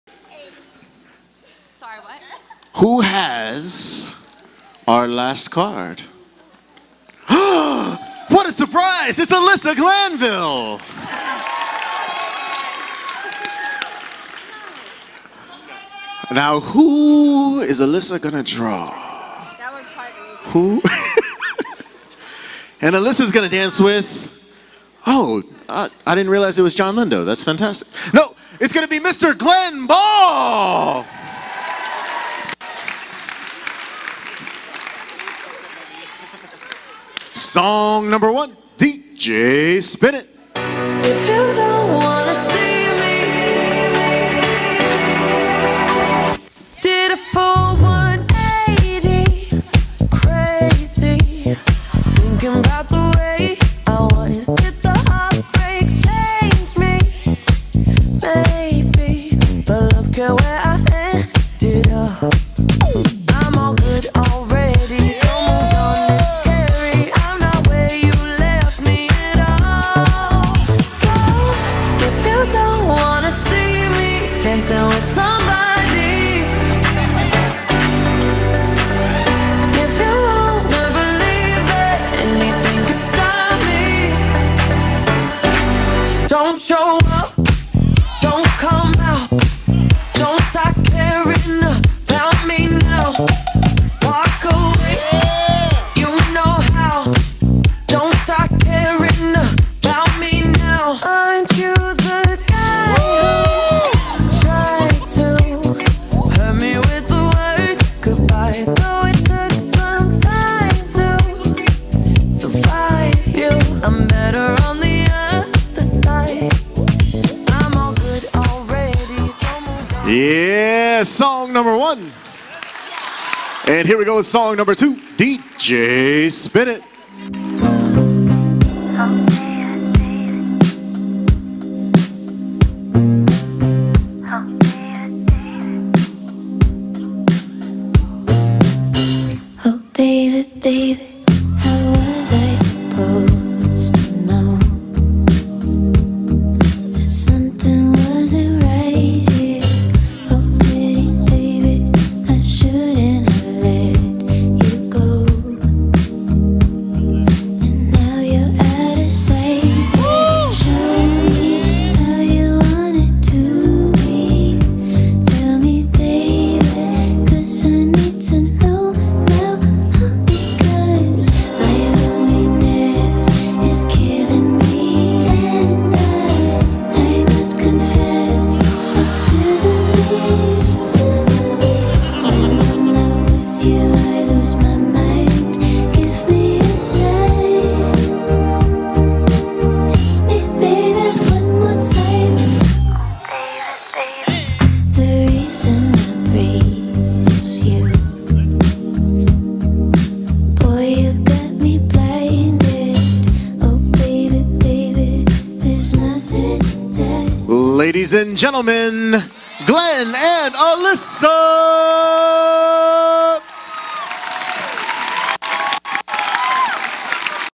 Alyssa Glanville + Glenn Ball Wild Wild Westie 2021 - Champions J&J - 3rd Place
Alyssa Glanville and Glenn Ball - West Coast Swing Improv at Wild Wild West 2021

Competition: Jack and Jill
Location: Dallas
Dancers: Alyssa & Glenn